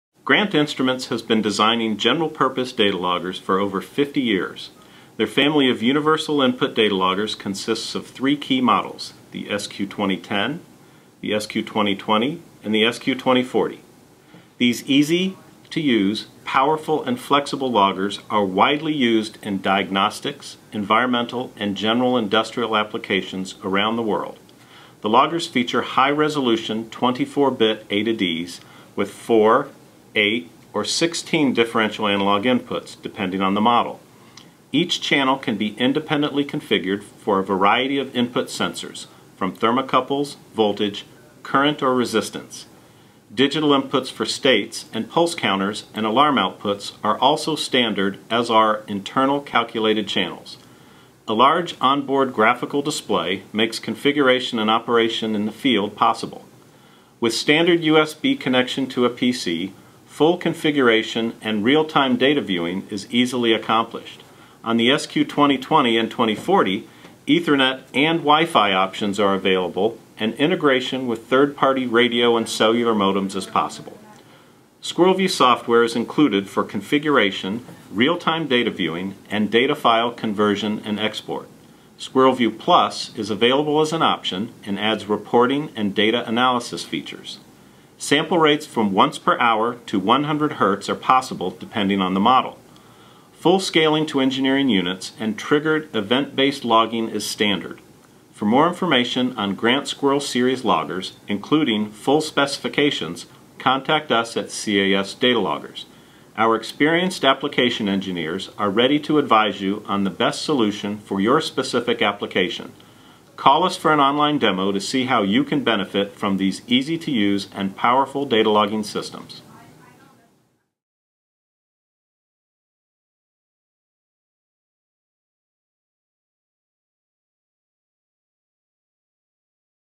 Introduction to the Grant Family of Portable Data Loggers
The Grant Squirrel 2010 is a versatile, portable datalogger with 4 to 8 universal analog input channels to measure current, voltage, humidity, resistance and temperature, plus 8 digital inputs. All inputs can be logged or used as triggers to automatically start or stop logging.

Grant’s SQ2020 series adds more alarm and pulse/counter inputs while storing up to 14 million readings. The advanced SQ2040 series has the same powerful features as the Squirrel 2020 series with additional high-speed logging on up to four channels and twice as many universal input channels. Great for high-speed vehicle testing or engine monitoring.